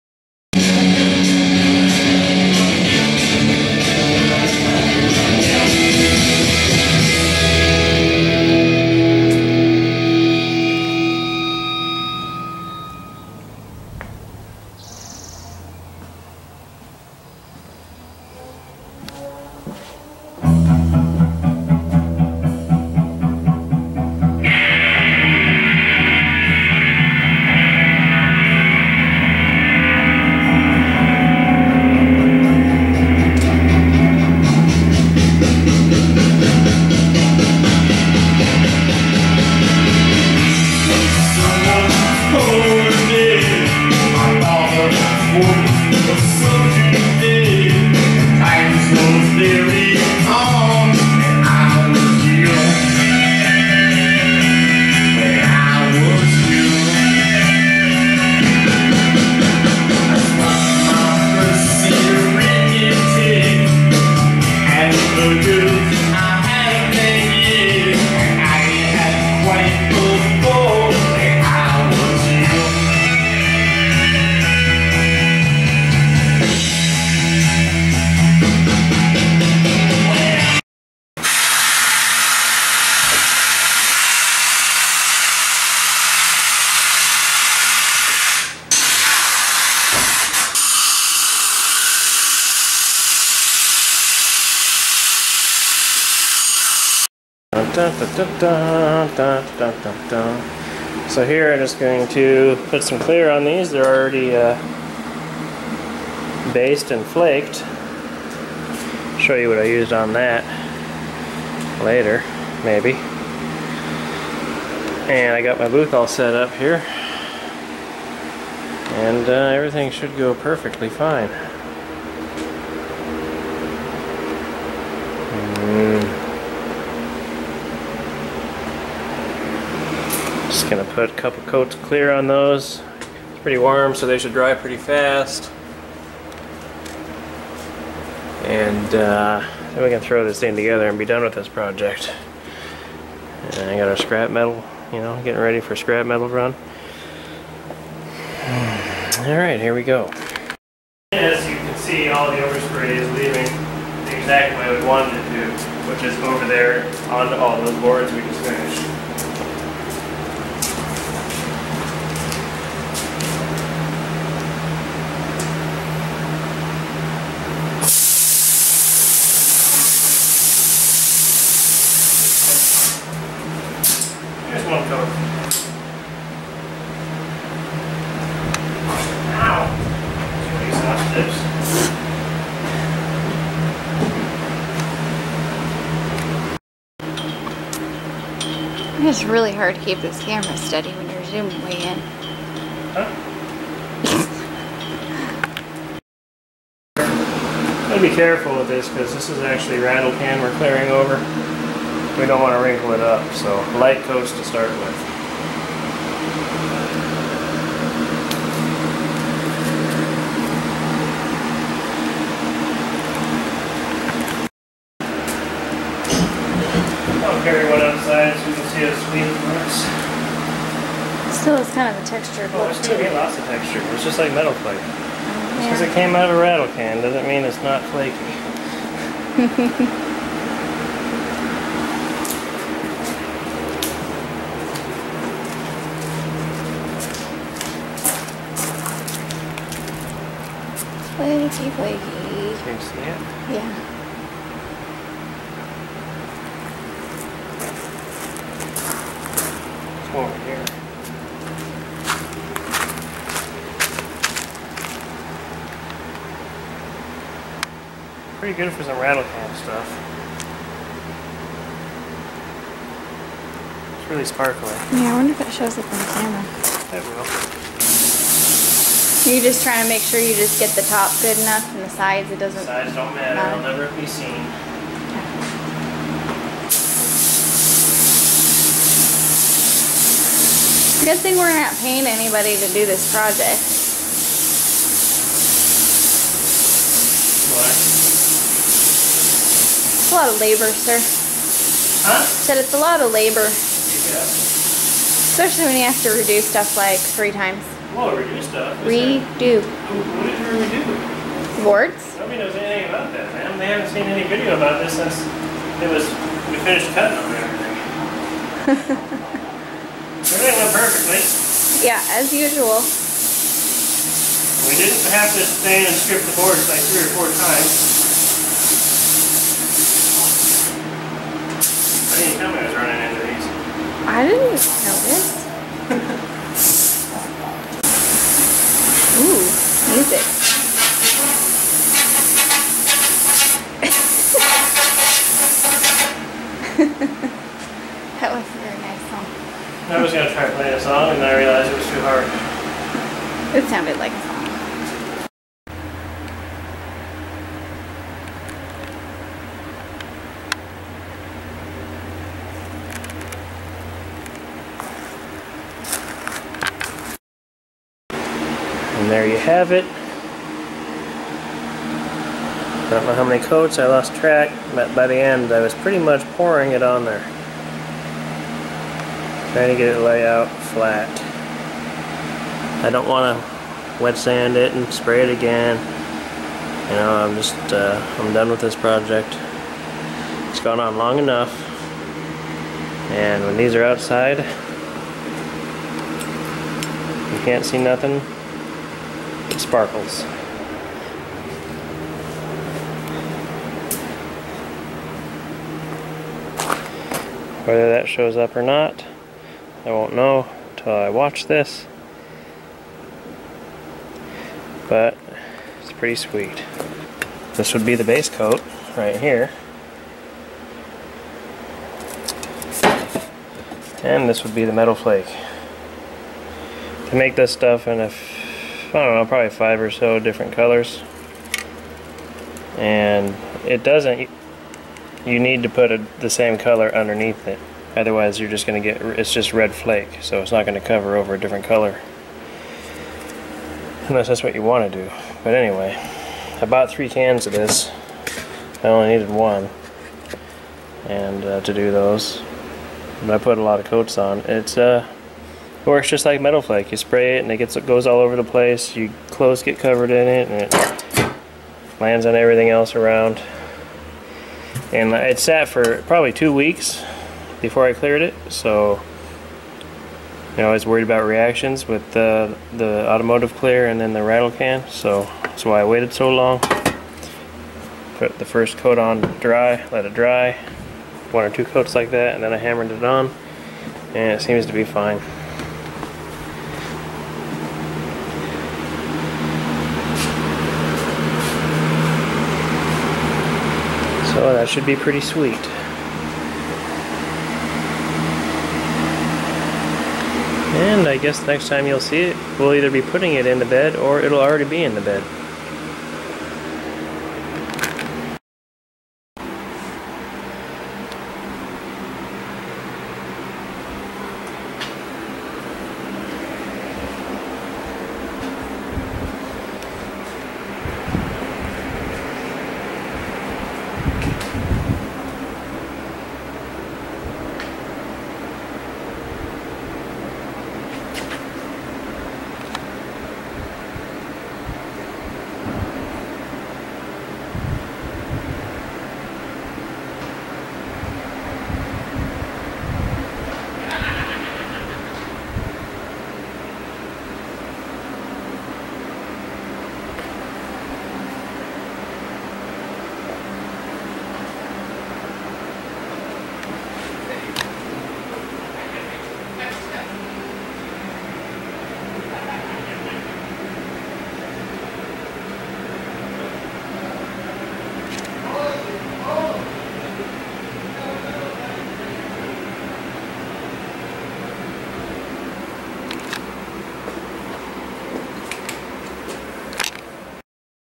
SYF: S10 bed floor done, Shoebox Ford project begining.
Maple bed floor with red metal flake rails.